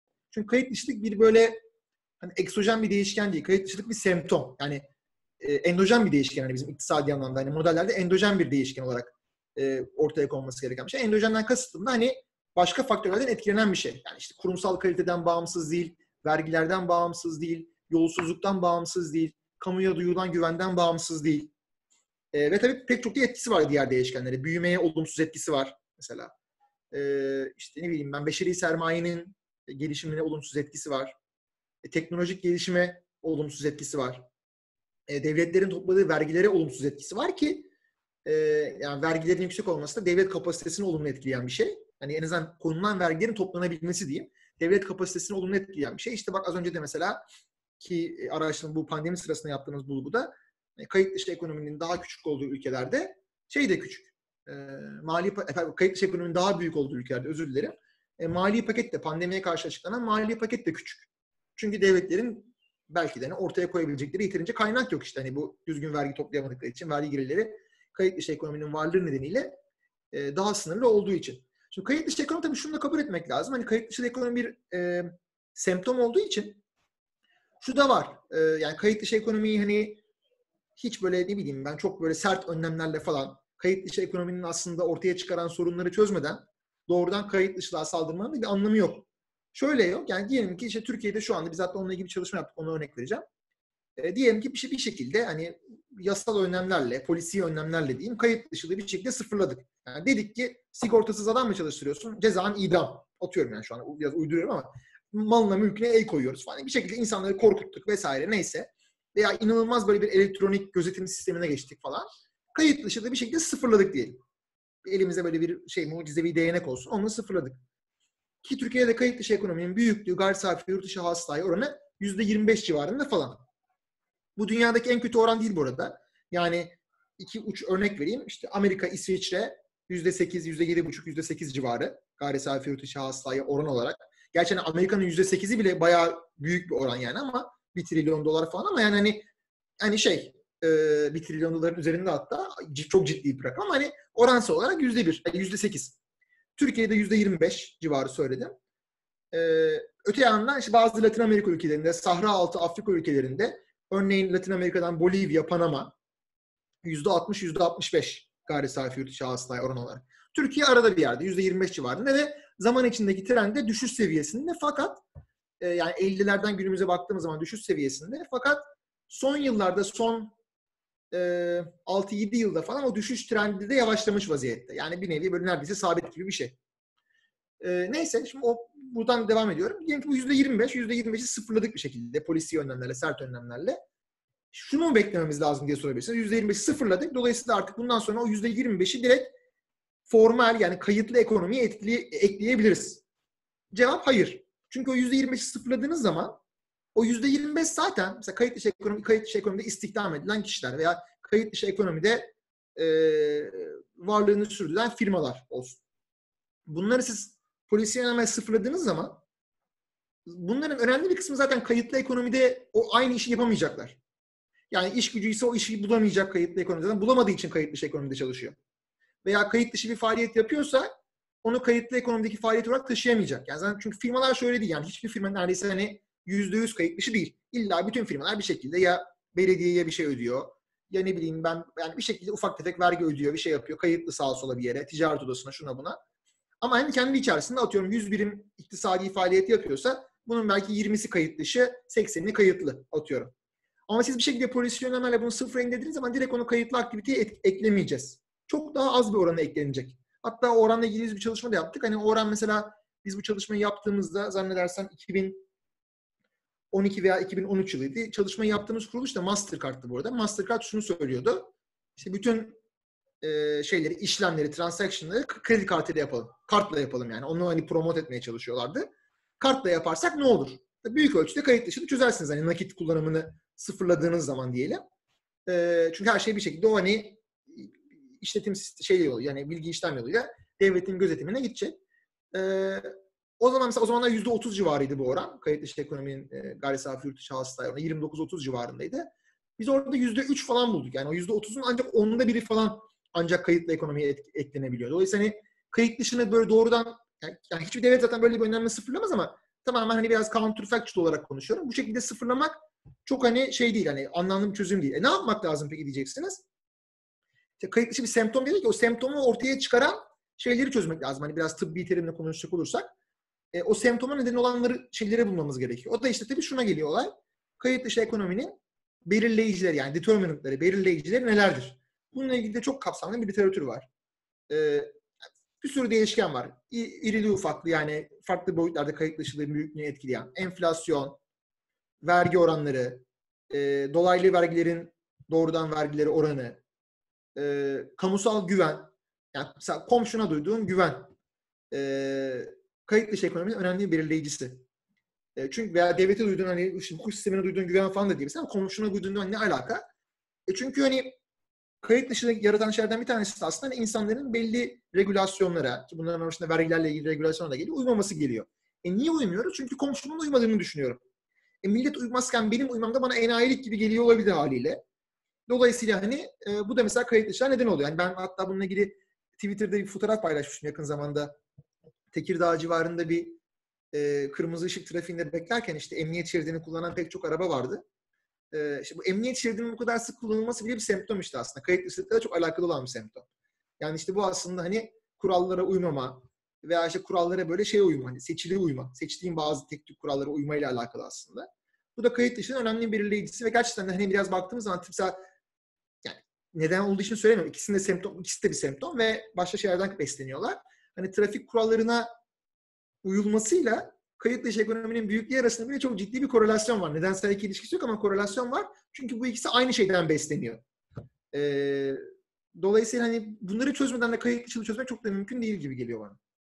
Prof. Dr. Ceyhun Elgin: "Türkiye'de komşuna duyduğun güven az, bu kanunlara uymakla bağlantılı."
Akademik Sohbetler (4. Bölüm) Konuk: Prof. Dr. Ceyhun Elgin Konu: Salgın Ekonomisi
Boğaziçi Üniversitesinden Prof. Dr. Ceyhun Elgin, Covid-19 pandemisinin dünyada yarattığı ekonomik krizin etkilerini, hükümetlerin ekonomik paketlerini, Türkiye ekonomisin salgın sürecindeki durumunu ve kayıtdışı ekonomi gibi birçok önemli konuyu değerlendirdi.

"Kayıtdışılık egzojen bir değişken olmaktan ziyade bir semptomdur. İktisadi anlamda ‘endojen’ bir değişken olarak görülür. Endojenden kasıt da başka faktörlerden etkilenen bir şey olmasıdır. Kurumsal kaliteden, vergilendirmeden, yolsuzluktan ve kamuya duyulan güvenden bağımsız olarak değerlendirilemez. Kayıtdışı ekonomi kavramı birçok faktörden etkilendiği gibi birçok faktörü de beraberinde etkilemektedir. Kayıtdışılığın büyümeye, beşeri sermayenin gelişimine, teknolojik gelişime veya devletlerin topladığı vergilere olumsuz etkileri var. Kayıtdışı ekonomide istihdam edilen işgücünü polisiye önlemlerle tamamen sıfırlasanız bile bu işgücünü formal ekonomiye entegre etmeniz mümkün değil. Kayıtdışı ekonomide istihdam edilen işgücü formal ekonomide aynı işi bulamayacak çünkü bu insanlar kayıtlı ekonomide iş bulamadığı için kayıtdışı ekonomide çalışıyorlar. Bu sorunu çözmek için kayıtdışı ekonomiyi bir semptom haline getiren nedenleri çözmemiz gerekiyor. Enflasyon, vergi oranları, dolaylı vergilerin doğrudan vergilere oranı ve kamusal güven gibi kayıtdışı ekonomiyi ortaya çıkaran belirleyici etkenleri tespit edip çözmek gerekiyor. Örnek olarak komşuya duyulan güven kayıtdışı ekonominin önemli bir belirleyicisidir. İnsanların, komşularının veya çevresindeki kişilerin devletin uyguladığı regülasyonlara uymaması aslında kayıtdışı ekonomiye karşı bir teşvik oluşturuyor. Emniyet şeridinin çok sık kullanılması bile kayıtdışı ekonomiyle çok alakalı bir semptom. Kurallara uymama veya kurallara seçili olarak uyma kayıtdışı ekonomi için belirleyici bir semptom oluşuruyor. Sonuç olarak da bu semptomları tedavi etmeden kayıtdışılık ile mücadele etmek çok da mümkün görünmüyor."